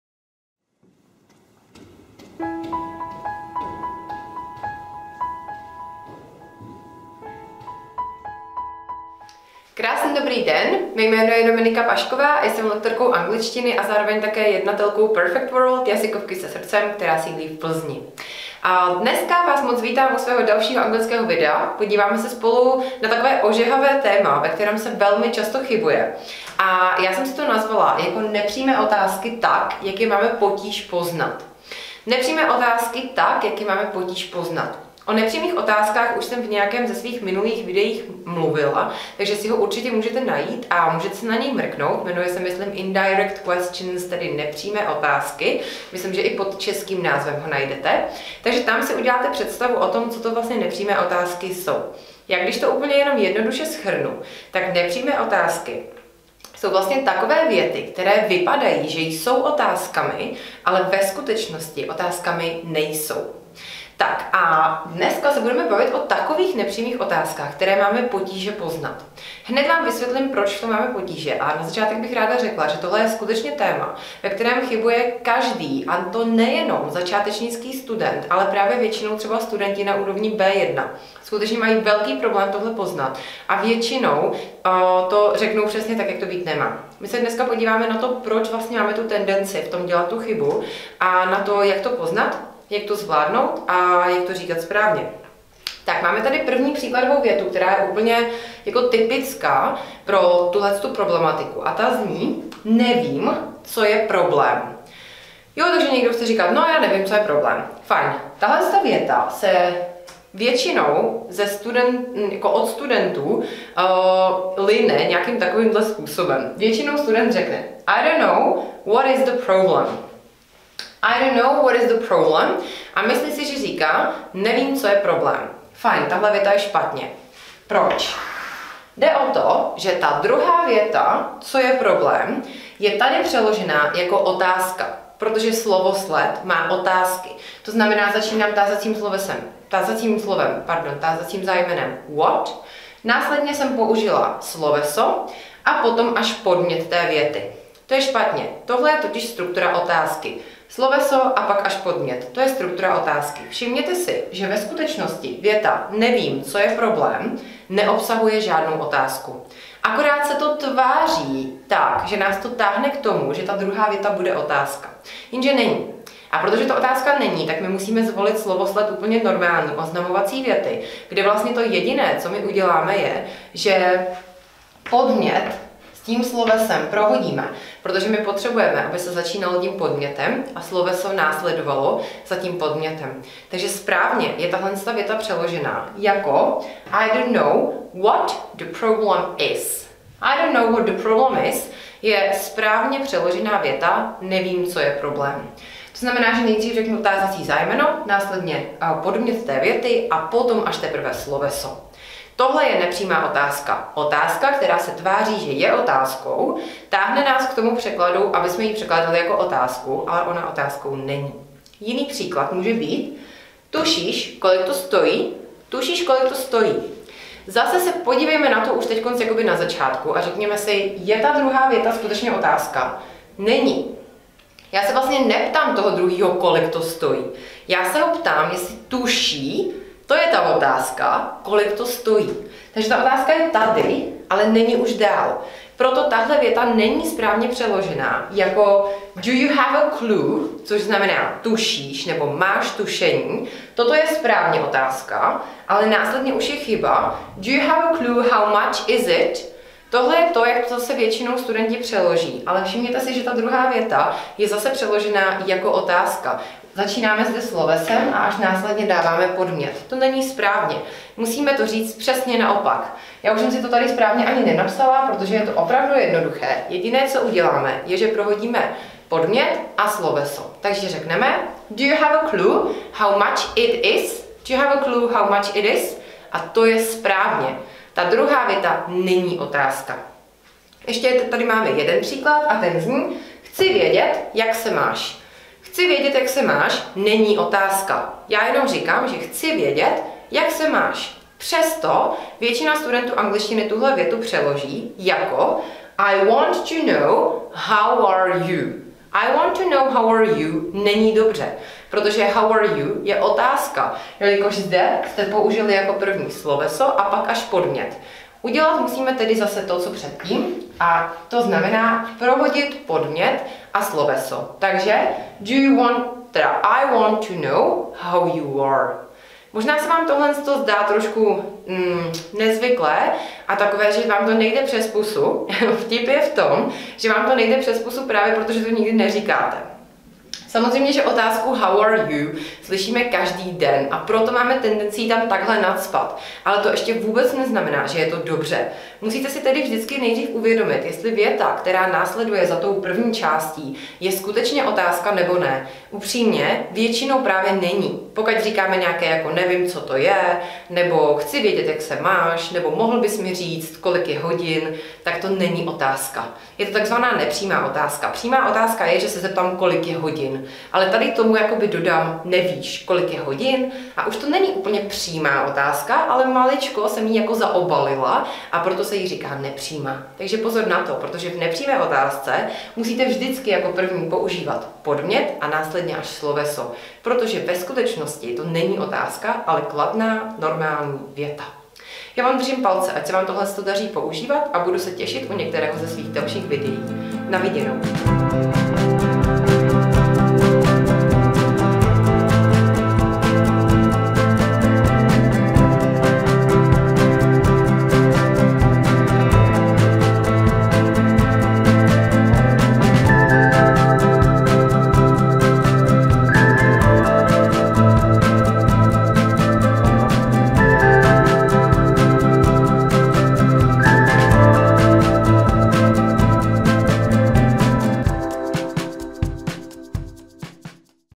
Nepřímé otázky v angličtině tak, jak je máme potíž poznat
Možná jste někdy v minulosti již slyšeli o anglických nepřímých otázkách - je to celkem obvyklá gramatika úrovně B1, která není nijak složitá. Jenže existuje několik případů, kdy se nám nepřímé otázky těžko rozpoznávají a dělá nám problém je správně přeložit. Mrkněte na následující video a zjistěte, které to jsou!